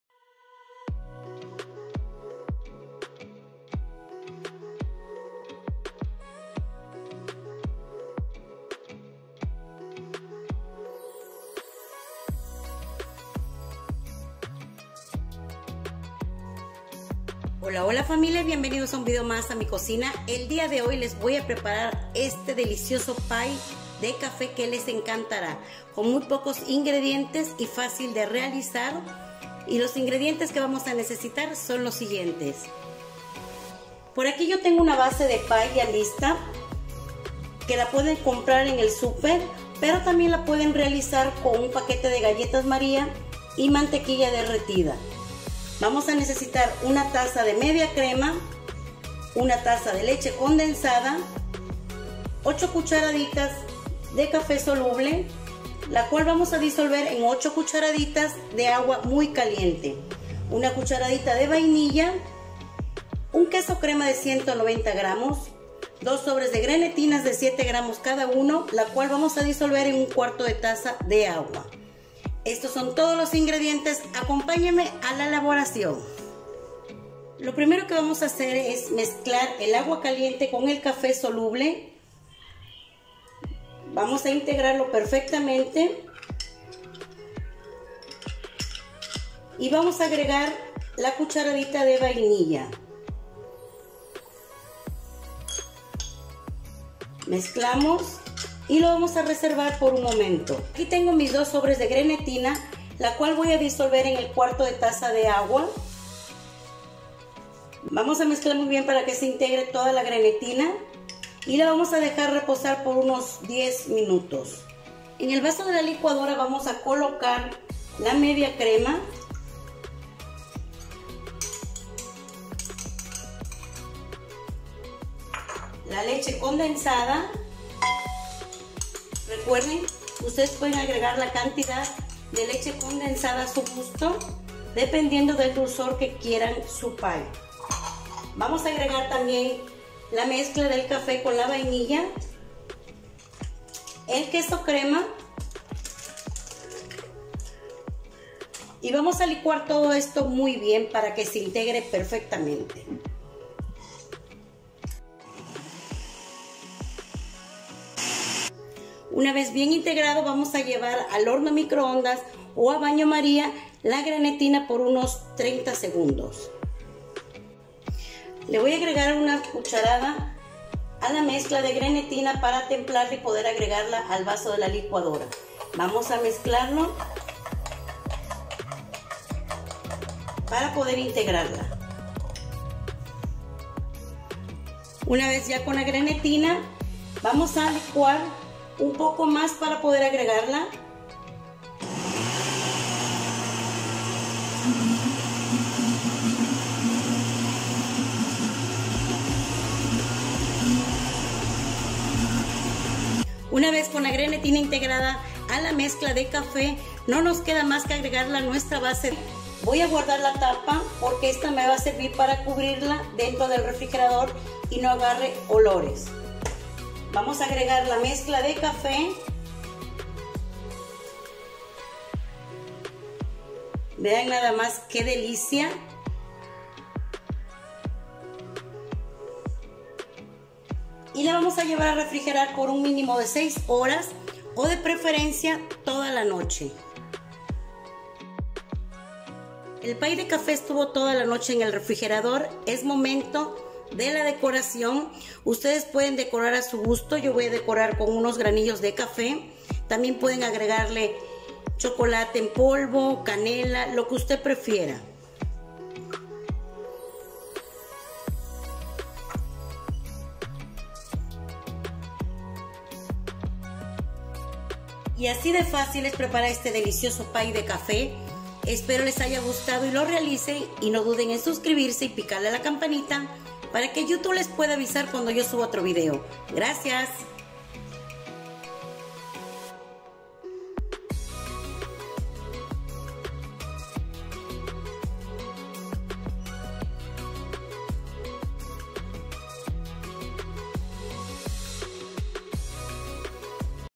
🥧PAY DE CAFE|riquísimo |Cocinando con hayde
Hola!!!
Bienvenidos a mi canal hoy  les prepare este delicioso postre a base de café, les encantará es muy fácil de realizar.

Ingredientes:
1 base para pay
8 cditas de café soluble
8 cditas de agua caliente
1 queso crema de 199 gr
1 cdita  de vainilla
2 sobres de grenetina
Disolverla en 1/4 de taza de agua y dejar reposar por 10 min.

Pará decorar
Siempre es a su gusto.
Yo decore, solo con Granillo de café